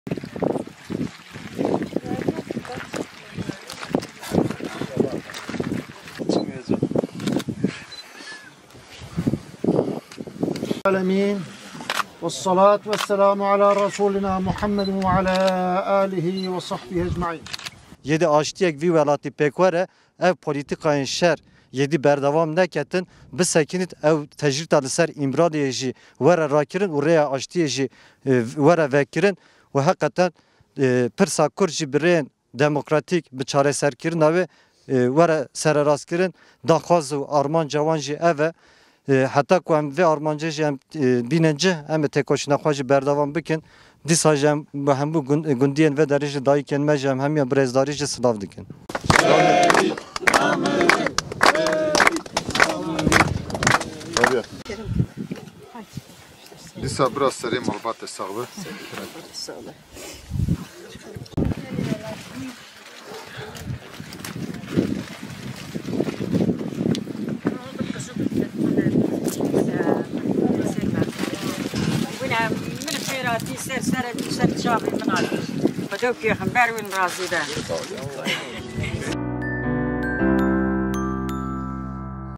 ÖZGÜRLÜK YÜRÜYÜŞÇÜLERİ CEYLAN ÖNKOL’U ANDI
PKK Lideri Abdullah Öcalan’ın fiziki özgürlüğü ve Kürt sorununun demokratik çözümü için 9 gündür yollarda olan Özgürlük Yürüyüşçüleri,  Ceylan Önkol’u mezarı başında andı